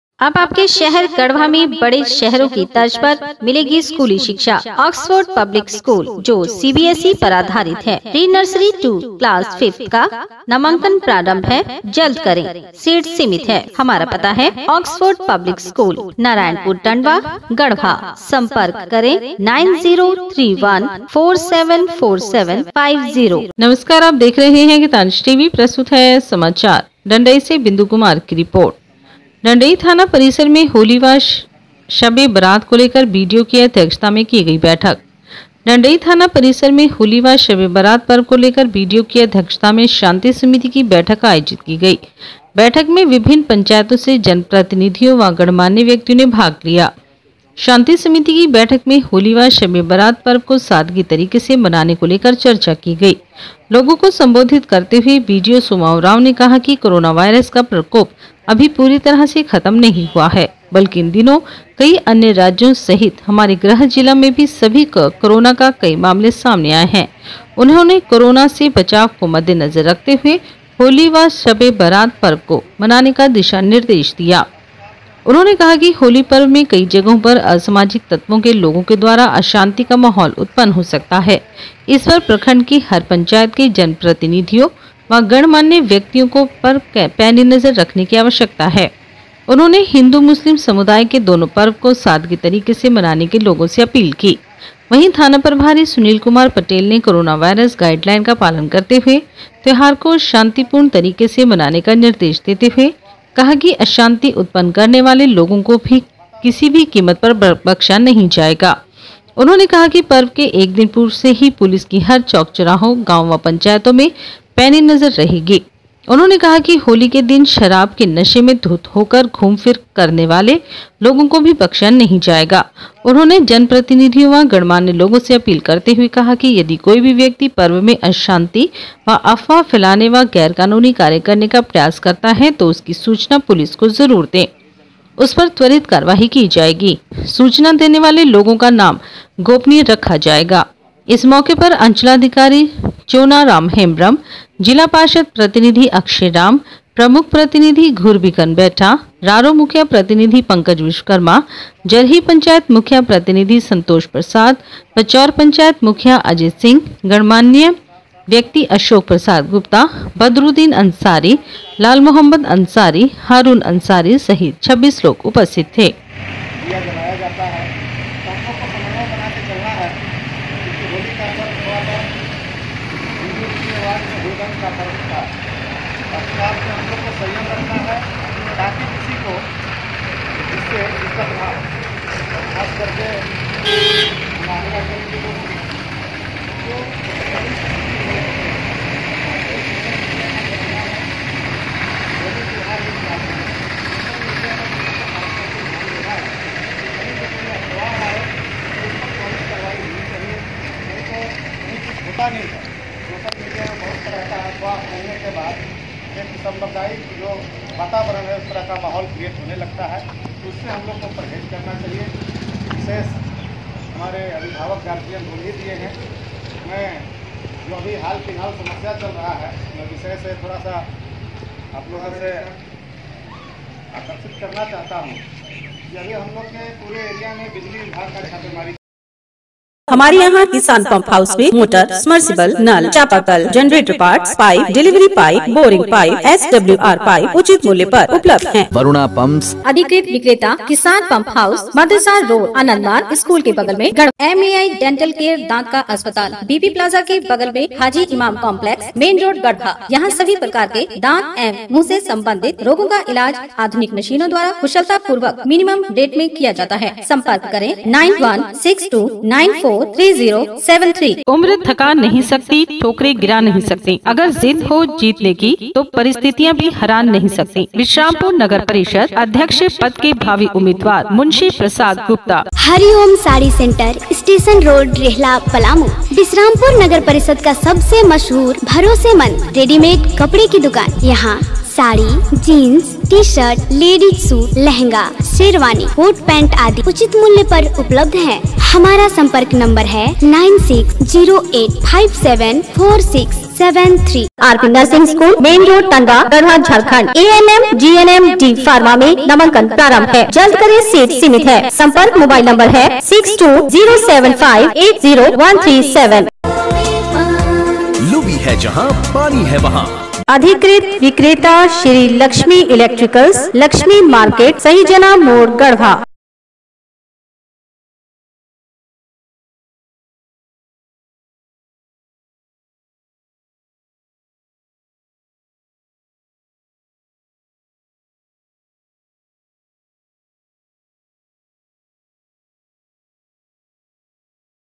डंडई थाना परिसर में होली व शब ए बरात को लेकर बीडीओ की अध्यक्षता में की गई बैठक,
डंडई थाना परिसर में होली व शबे बरात पर्व को लेकर बीडीओ की अध्यक्षता में शांति समिति की बैठक आयोजित की गई। बैठक में विभिन्न पंचायतों से जनप्रतिनिधियों व गणमान्य व्यक्तियों ने भाग लिया। शांति समिति के बैठक में होली व शबे बरात पर्व को  सादगी  तरीके से  मनाने को लेकर चर्चा की गई । लोगों को संबोधित करते हुए बीडीओ सोमा उरांव ने कहा कि कोरोनावायरस का प्रकोप अभी पूरी तरह से खत्म नहीं हुआ है। बल्कि इन दिनों कई अन्य राज्यों सहित हमारे गृह जिला में भी कोरोना का कई  मामले सामने आया है। उन्होंने कोरोना से बचाव को मध्य नजर रखते हुए होली व सब ए बरात पर्व को मनाने का दिशा निर्देश दिया । उन्होंने कहा कि होली पर्व में कई जगहों पर असामाजिक तत्वों के लोगों द्वारा अशांति का माहौल उत्पन्न हो सकता है। इस पर प्रखंड के हर पंचायत के जनप्रतिनिधियों व गणमान्य व्यक्तियों को पैनी नजर रखने की आवश्यकता है। उन्होंने  हिंदू मुस्लिम समुदाय के दोनों पर्व को सादगी तरीके से मनाने का लोगों से अपील किया। वहीं थाना प्रभारी सुनील कुमार पटेल ने  कोरोनावायरस गाइडलाइन का पालन करते हुए  त्यौहार को शांतिपूर्ण तरीके से मनाने का निर्देश देते हुए कहा कि अशांति उत्पन्न करने वाले लोगों को किसी भी कीमत पर बख्शा नहीं जाएगा। उन्होंने कहा कि पर्व के 1 दिन पूर्व से ही पुलिस की हर चौक चौराहों, गांव व पंचायतों में पैनी नजर रहेगा। उन्होंने कहा कि होली के दिन शराब के नशे में धुत होकर घूम फिर करने वाले लोगों को भी बख्शा नहीं जाएगा उन्होंने जनप्रतिनिधियों व गणमान्य लोगों से अपील करते हुए कहा कि यदि कोई भी व्यक्ति पर्व में अशांति व अफवाह फैलाने व  गैर कानूनी कार्य करने का प्रयास करता है तो उसकी सूचना पुलिस को  जरूर दें। उस पर त्वरित कार्रवाई की जाएगी। सूचना देने वाले लोगों का नाम गोपनीय रखा जाएगा। इस मौके पर  अंचलाधिकारी चोना राम हेंब्रम,  जिला पार्षद प्रतिनिधि अक्षय राम, प्रमुख प्रतिनिधि घुरबिगन बैठा, रारो मुखिया प्रतिनिधि पंकज विश्वकर्मा ,जरही पंचायत मुखिया प्रतिनिधि संतोष प्रसाद, पचौर पंचायत  मुखिया अजय सिंह  गणमान्यय व्यक्ति अशोक प्रसाद गुप्ता, बदरुद्दीन अंसारी,लाल मोहम्मद अंसारी, हारुन अंसारी  सहित 26 लोग उपस्थित थे।